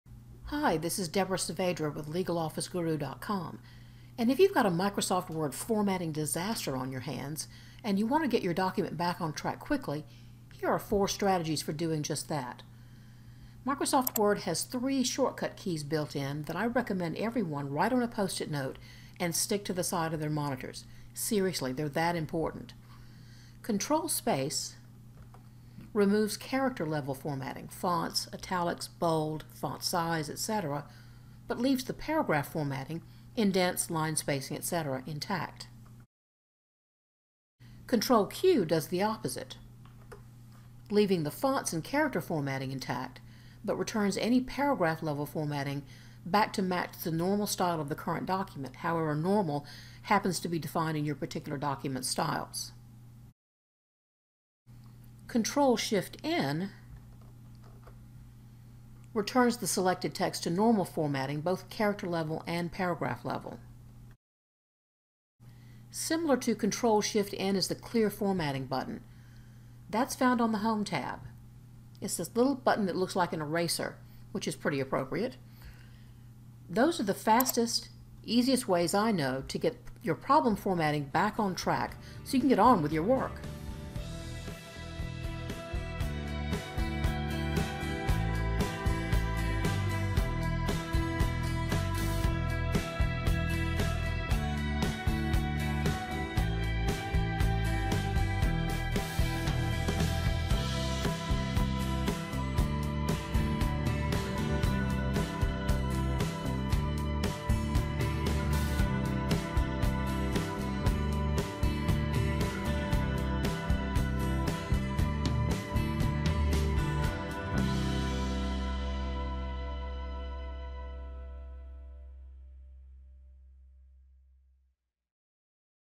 The 4 fastest ways to fix msword formatting
If you're in a hurry to get your Microsoft Word document out the door, and your formatting just isn't responding to your best efforts to fix it, here are three shortcut keys and a Ribbon button that will save your life!

Frustrated with automatic paragraph numbering? Make your paragraph numbering bulletproof with my free, no-sign-up-required online course